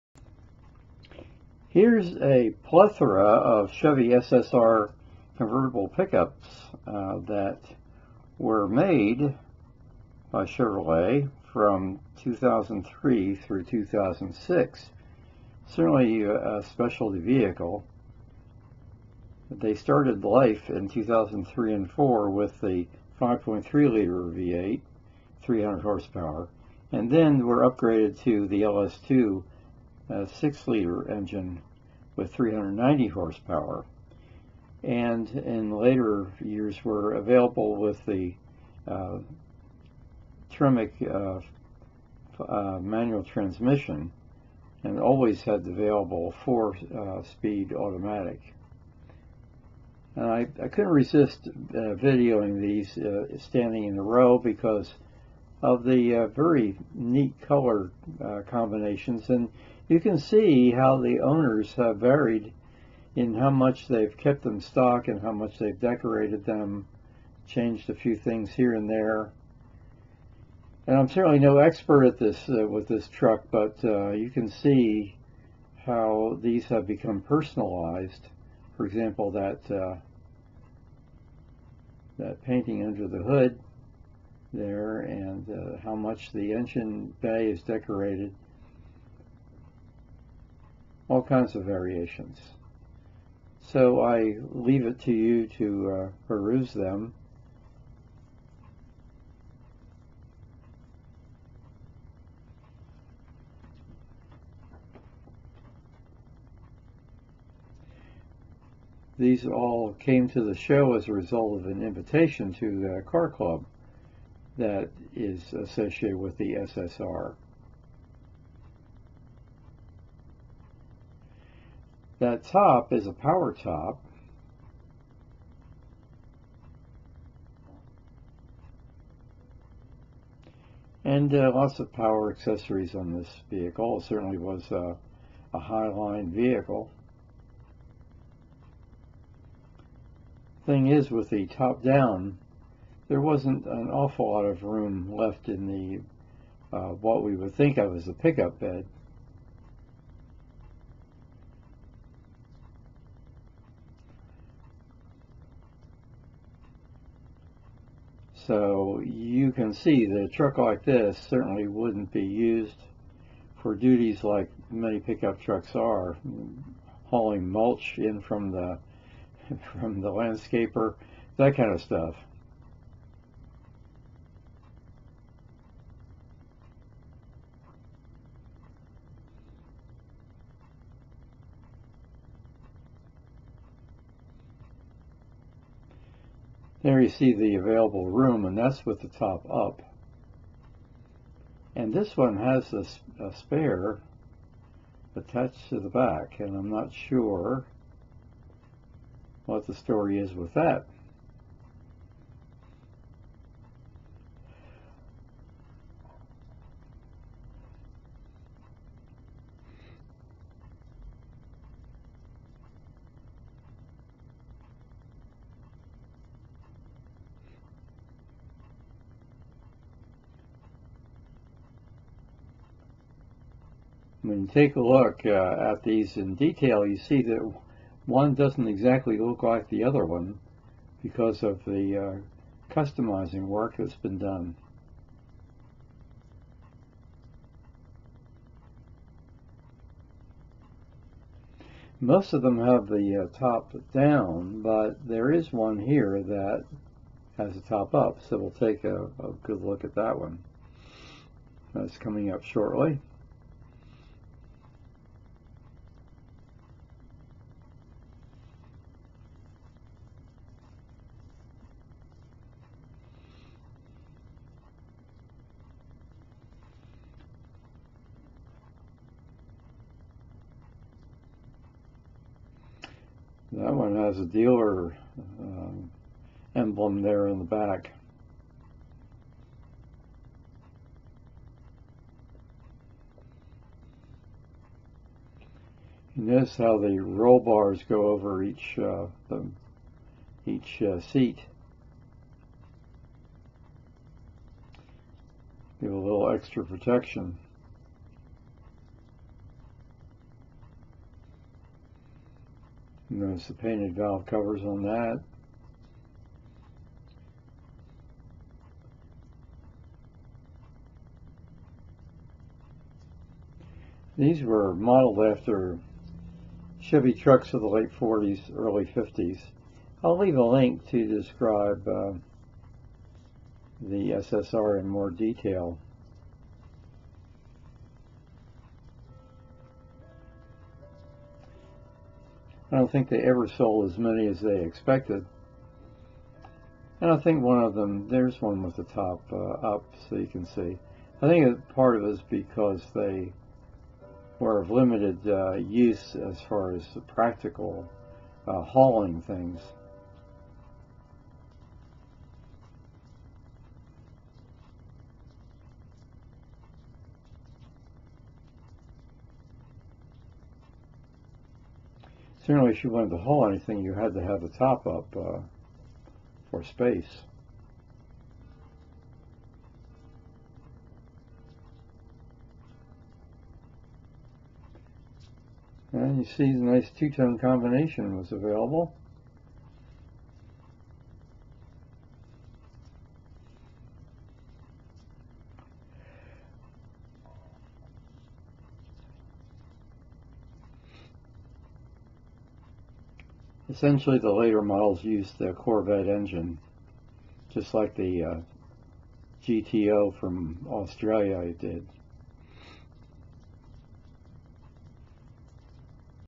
2003-2006 Chevy SSR Convertible Pickups Eustis0924226240
Ford had their revisited Thunderbird, an exercise in retro styling' and Chrysler had the PT Cruiser and Prowler, so Chevy was following a trend when it engineered the SSR convertible pickup.  Dodge also had a convertible Dakota truck.  Chevy sold quite a few, but unfortunately it didn't meet sales expectations, and it's my feeling that the truck bed was never a serious hauler of cargo.  Here's Wikipedia's take on the vehicle

Show less